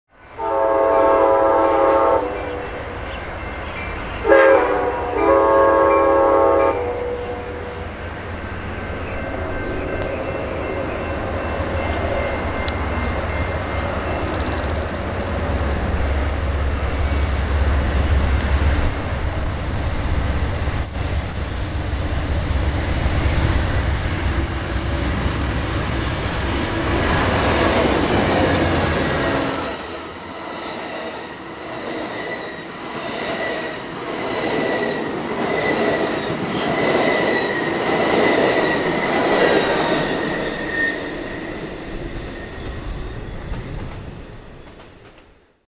Amtrak Train 6 08jul20
Amtrak Train #6 California Zephyr passes hopper cars full of old railroad ties on the Pinole siding. Train #6 is out of the chute OT this morning as it heads for it's next stop of Martinez, CA.